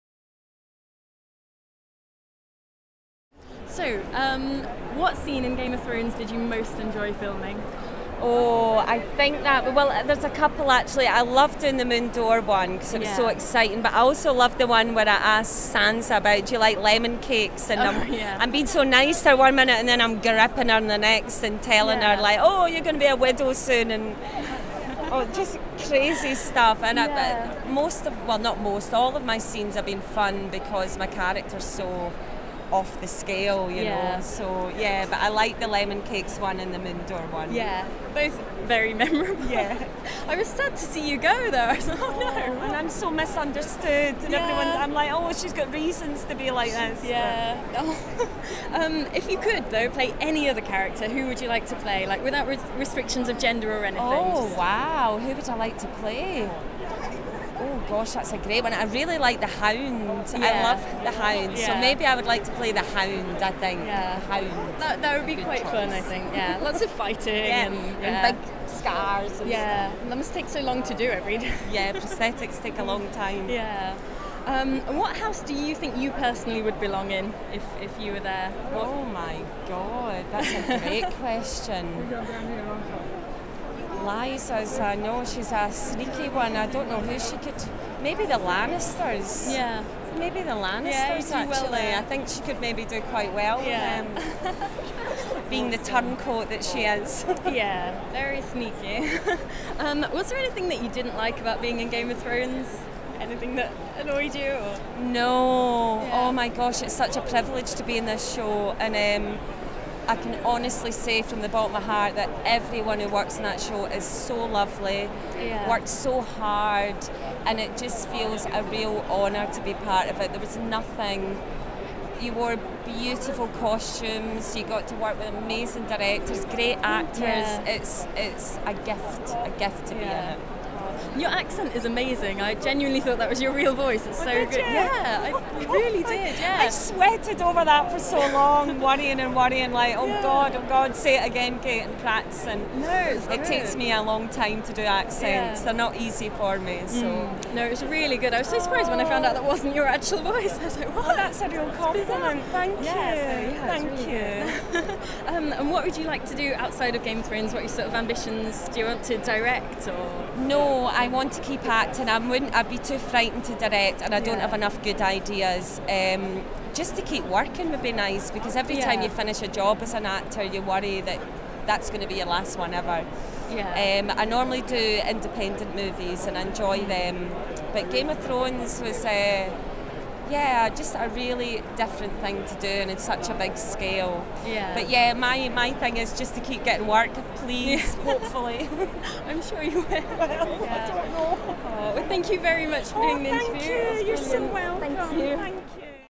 LFCC 2014: A Place to Hang Your Cape's Interview with Kate Dickie (Lysa Arryn on Game of Thrones)
Presenter: Grace Davis
Camera: Aidan Davis
Editing: Peter Collins and Grace Davis

for more great videos, reviews and interviews!